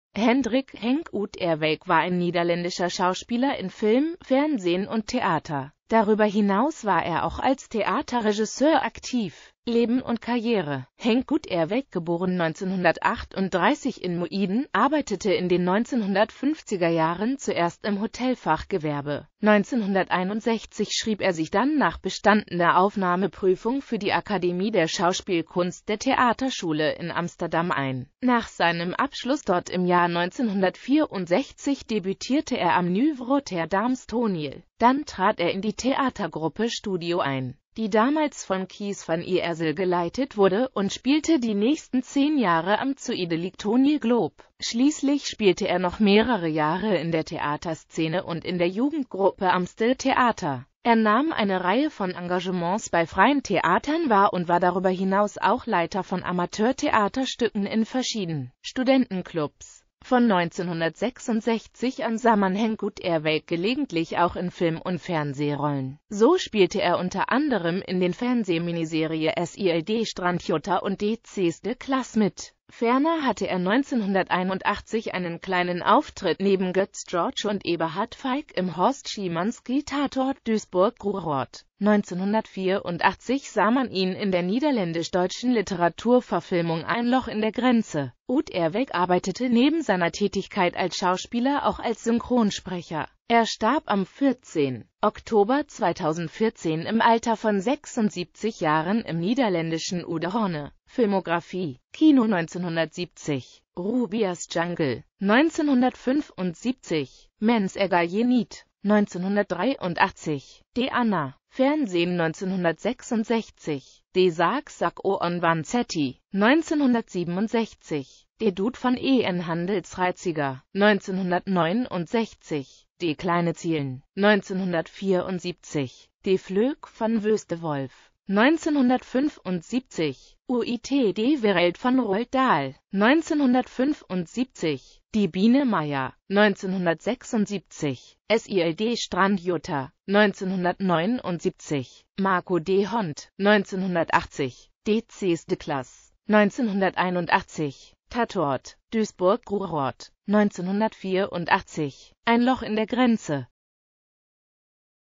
Henk Uterwijk
Vielen Dank für Ihre Unterstützung:

Hendrik „Henk“ Uterwijk (* 24.August 1938 in Muiden; † 14.Oktober 2014 in Oudehorne) war ein niederländischer Schauspieler in Film, Fernsehen und Theater.Darüber hinaus war er auch als Theaterregisseur aktiv.

Bild-Copyright-Informationen
Urheber Info: Serge-Henri Valcke